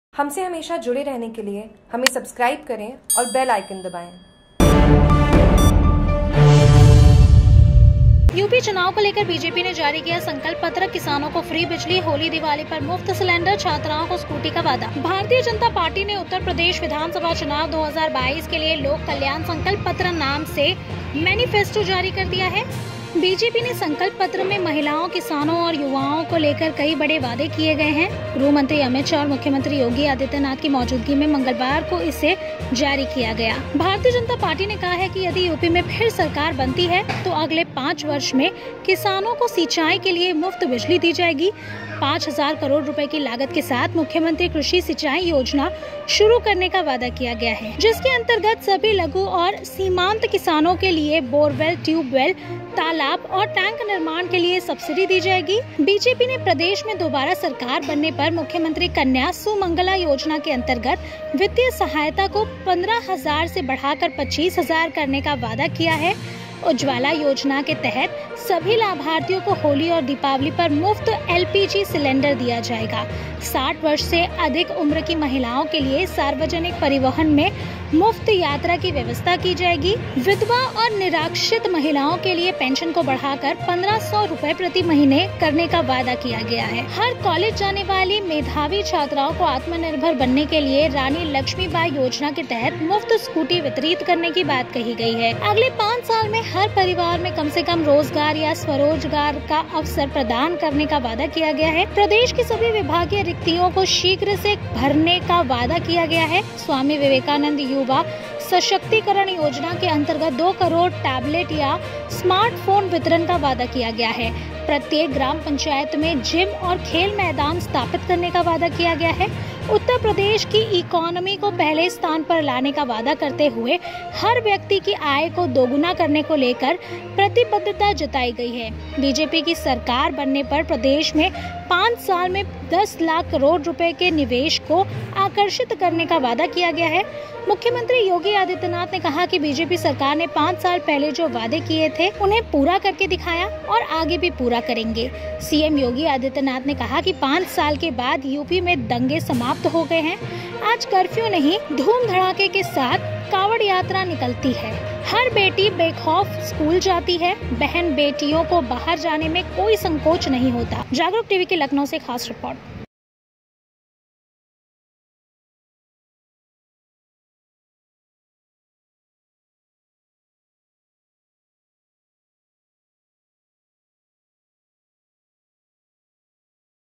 उत्तर प्रदेश विधानसभा चुनाव 2022 के लिए लोक कल्याण संकल्प पत्र नाम से मेनिफेस्टो जारी कर दिया है
भारतीय जनता पार्टी ने उत्तर प्रदेश विधानसभा चुनाव 2022 के लिए लोक कल्याण संकल्प पत्र नाम से मेनिफेस्टो जारी कर दिया है। बीजेपी के संकल्प पत्र में महिलाओं, किसानों और युवाओं को लेकर कई बड़े वादे किए गए हैं। गृहमंत्री अमित शाह और मुख्यमंत्री योगी आदित्यनाथ की मौजूदगी में मंगलवार को इसे जारी किया गया।

भारतीय जनता पार्टी ने कहा है कि यदि यूपी में फिर सरकार बनती है तो अगले 5 वर्ष में किसानों को सिंचाई के लिए मुफ्त बिजली दी जाएगी। 5 हजार करोड़ रुपए की लागत के साथ मुख्यमंत्री कृषि सिंचाई योजना शुरू करने का वादा किया गया है। जिसके अंतर्गत सबी लघु और सीमांत किसानों के लिए बोरवेल ट्यूबवेल, तालाबा और टैंक निर्माण के लिए सब्सिडी दी जाएगी।

बीजेपी ने प्रदेश में दोबारा सरकार बनने पर मुख्यमंत्री कन्या सुमंगला योजना के अंतर्गत वित्तीय सहायता को 15 हजार से बढ़ाकर 25 हजार करने का वादा किया है। उज्ज्वला योजना के तहत सभी लाभार्थियों को होली और दीपावली पर मुफ्त एलपीजी सिलेंडर दिया जाएगा। 60 वर्ष से अधिक उम्र की महिलाओं के लिए सार्वजनिक परिवहन में मुफ्त यात्रा की व्यवस्था की जाएगी। विधवा और निराश्रित महिलाओं के लिए पेंशन को बढ़ाकर 1500 रुपए प्रति महीने करने का वादा किया गया है। हर कॉलेज जाने वाली मेधावी छात्राओं को आत्मनिर्भर बनाने के लिए रानी लक्ष्मी बाई योजना के तहत मुफ्त स्कूटी वितरित करने की बात कही गई है।

अगले पांच साल में हर परिवार में कम से कम रोजगार या स्वरोजगार का अवसर प्रदान करने का वादा किया गया है। प्रदेश की सभी विभागीय रिक्तियों को शीघ्रता से भरने का वादा किया गया है। स्वामी विवेकानंद युवा सशक्तिकरण योजना के अंतर्गत 2 करोड़ टैबलेट या स्मार्टफोन वितरण का वादा किया गया है। प्रत्येक ग्राम पंचायत में जिम और खेल मैदान स्थापित करने का वादा किया गया है। उत्तर प्रदेश की इकॉनमी को पहले स्थान पर लाने का वादा करते हुए हर व्यक्ति की आय को दोगुना करने को लेकर प्रतिबद्धता जताई गई है। बीजेपी की सरकार बनने पर प्रदेश में 5 साल में 10 लाख करोड़ रुपए के निवेश को आकर्षित करने का वादा किया गया है।